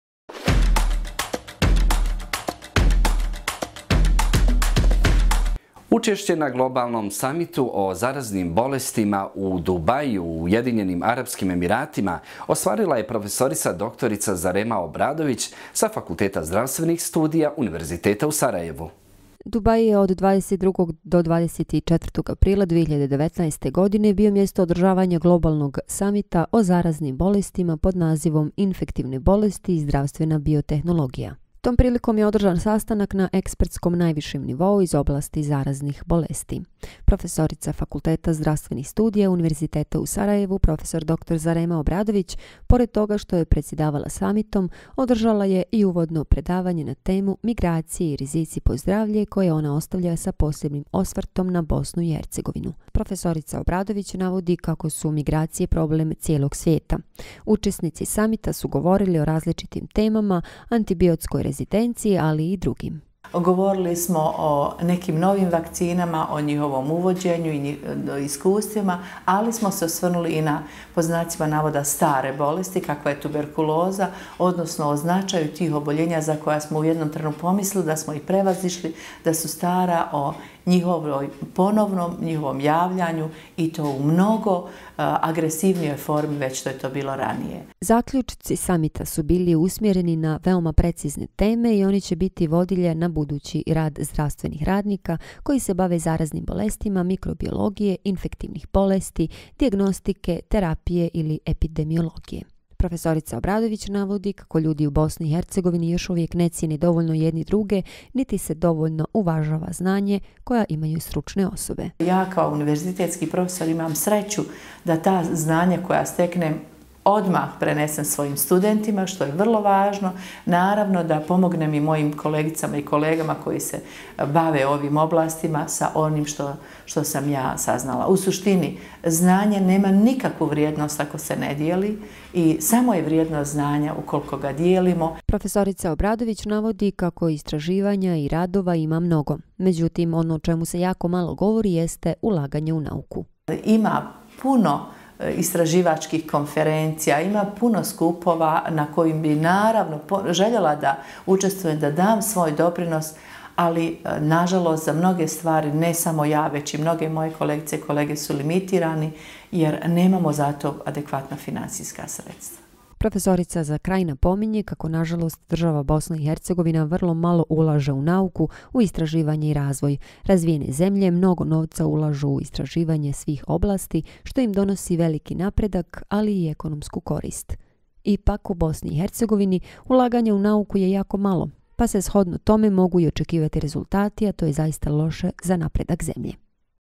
Prof. dr. Zarema Obradović presjedavala globalnim samitom u Dubaiju - Impuls znanja 93
Dubai je od 22-24. aprila 2019. godine bio mjesto održavanja globalnog samita o zaraznim bolestima pod nazivom „Infektivne bolesti i zdravstvena biotehnologija“. Tom prilikom je održan sastanak na ekspertskom najvišem nivou iz oblasti zaraznih bolesti. Profesorica Fakulteta zdravstvenih studija Univerziteta u Sarajevu prof. dr. Zarema Obradović pored toga što je presjedavala samitom, održala je i uvodno predavanje na temu: Migracije i rizici po zdravlje koje ona ostavlja sa posebnim osvrtom na Bosnu i Hercegovinu.